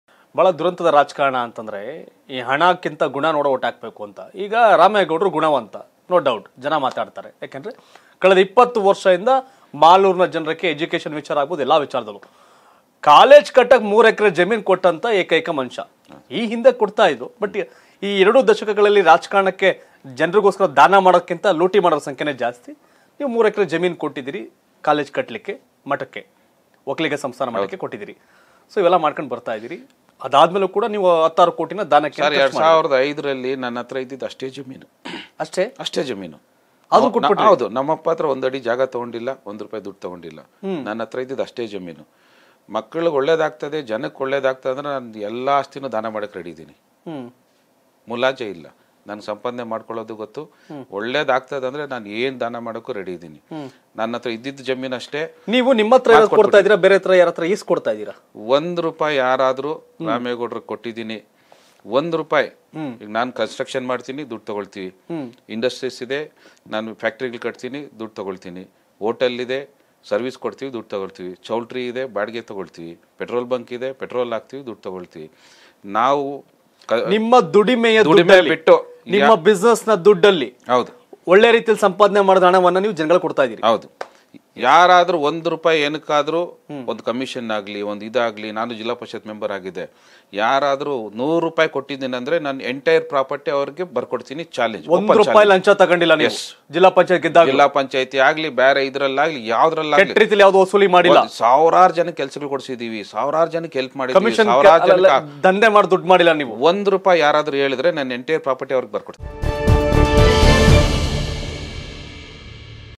Malur | ನಂಗೆ ಯಾರಾದ್ರೂ 1 ರೂಪಾಯಿ ಲಂಚ ಕೊಟ್ಟಿದ್ರೆ, ನನ್ನ ಆಸ್ತಿ ಬರ್ಕೊಡ್ತೀನಿ.! JDS Ramegowda | KarnatakaTV
Malur | ನಂಗೆ ಯಾರಾದ್ರೂ 1 ರೂಪಾಯಿ ಲಂಚ ಕೊಟ್ಟಿದ್ರೆ, ನನ್ನ ಆಸ್ತಿ ಬರ್ಕೊಡ್ತೀನಿ.! JDS Ramegowda | Karnataka TV